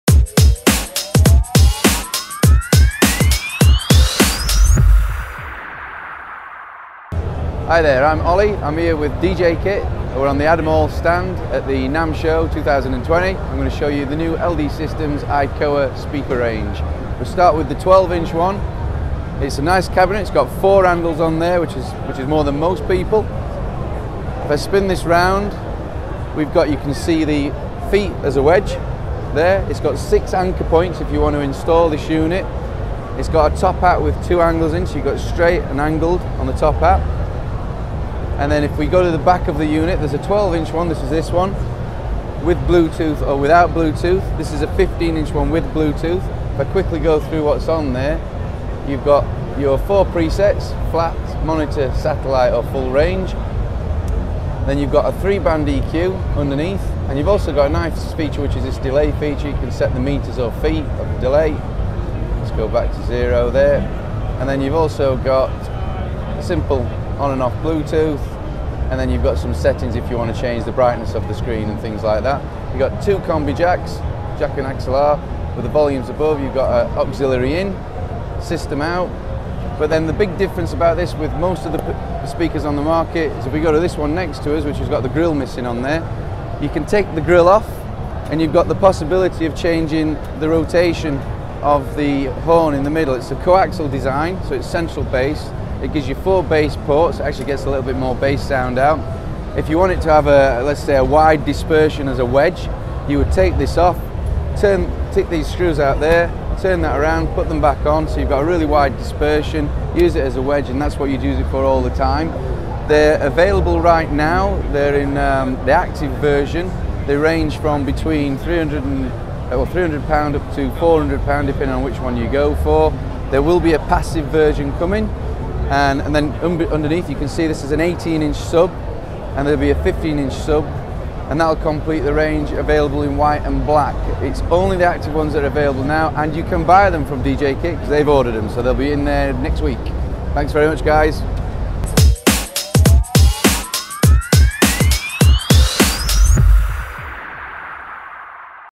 First look at the LD Systems ICOA speaker range @ NAMM 2020 - djkit.tv
The LD ICOA is a compact, active PA speaker which can be used in either horizontal or vertical applications thanks to its' unique rotating horn placed in the center of the speaker.
The onboard DSP lets you select from 4 presets as well as 3 band EQ and delay options.You can use the ICOA on its own  for smaller applications thanks to the two balanced inputs with level control and a 3.5mm aux input.